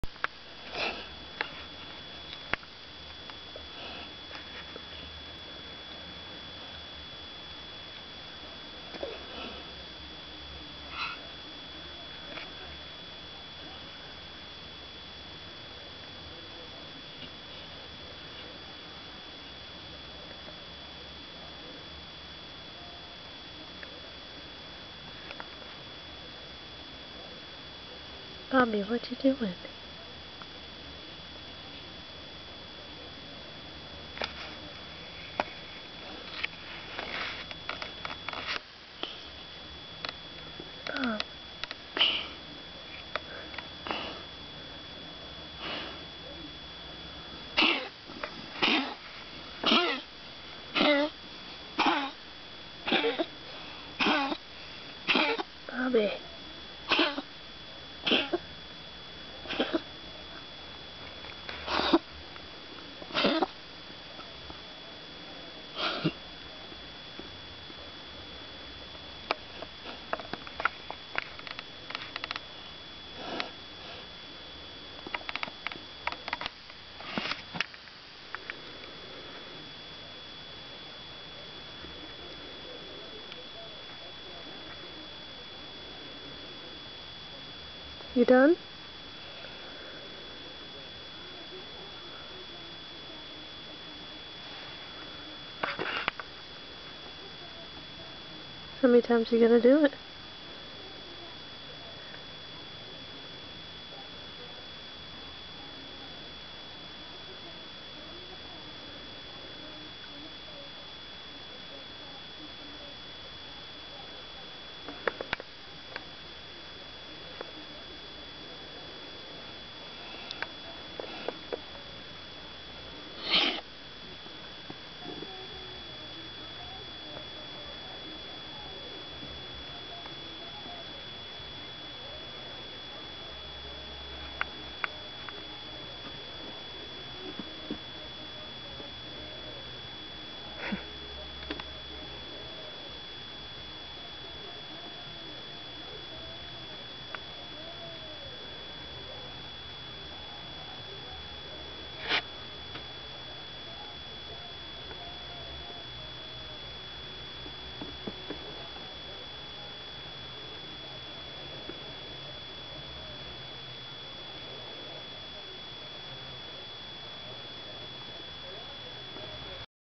Feline Auto-Erotic Asphyxiation (Bob)
This is Silent Bob in 2006 doing his thing. He enjoys choking himself whenever he gets the chance.